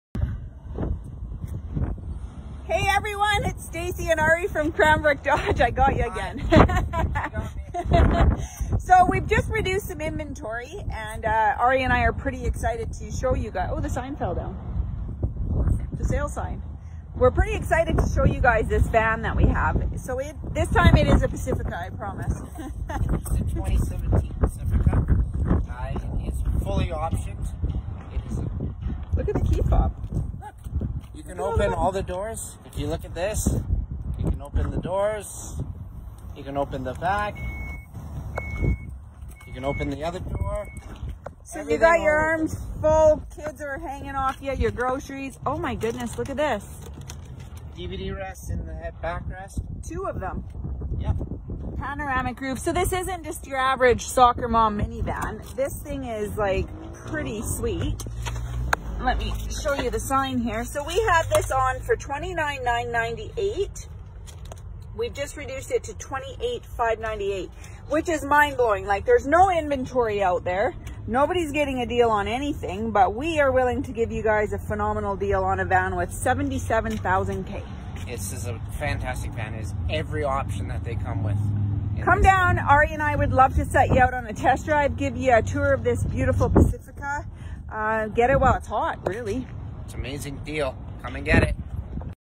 Chrysler Pacifica
Stacey and Ari show us this preowned Chrysler Pacifica - not your average soccer mom van!